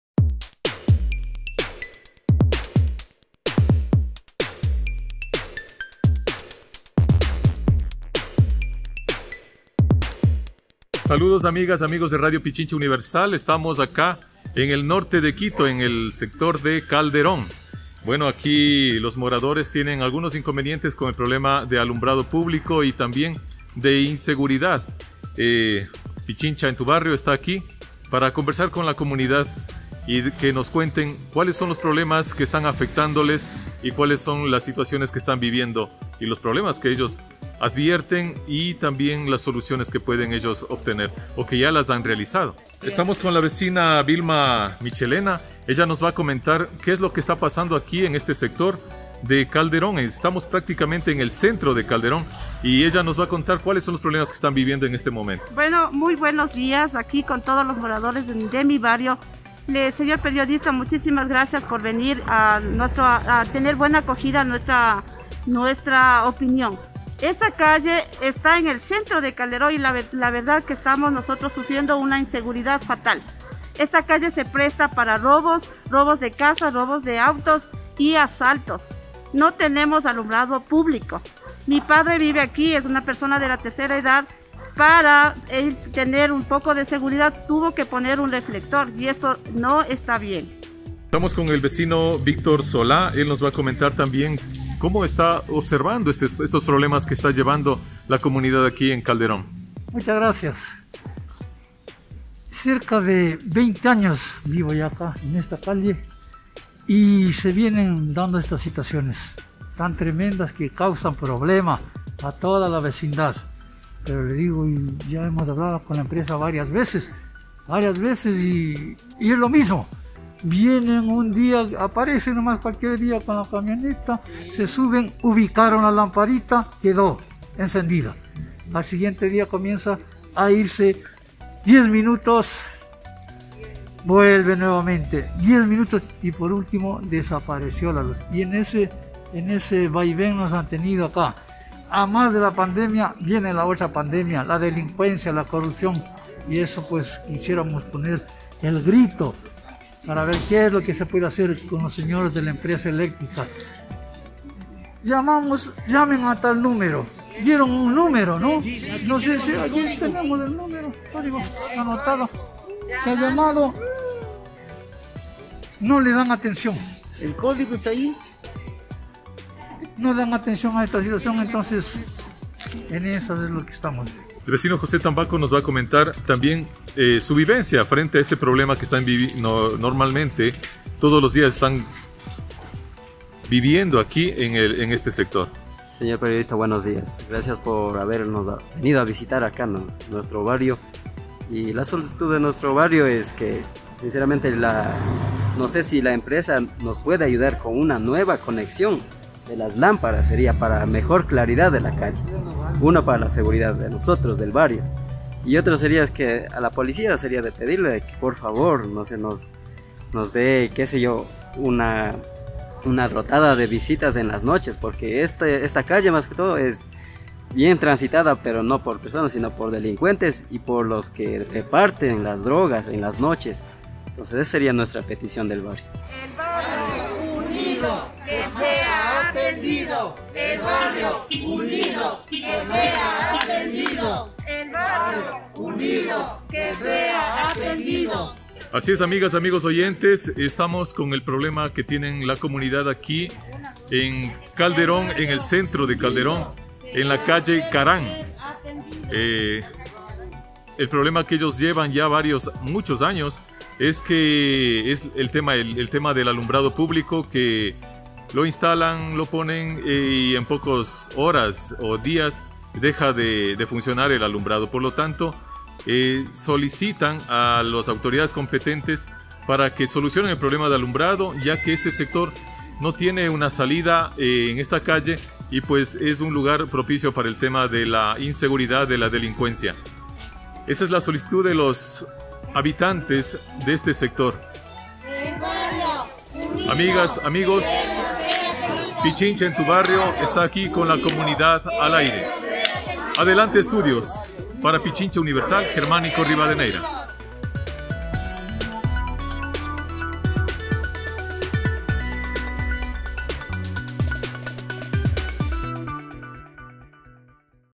Pichincha en Tu Barrio | Moradores de la calle Caran en Calderón piden alumbrado público y seguridad
Los vecinos de la calle Caran en el centro de Calderón, al norte de Quito, denuncian problemas con el alumbrado público, desde hace varios años. Esta situación es el ambiente propicio para los asaltos, robos de vehículos y consumo de droga, por lo que exigen a las autoridades de la Empresa eléctrica, así como de la Policía Nacional, soluciones a sus problemas.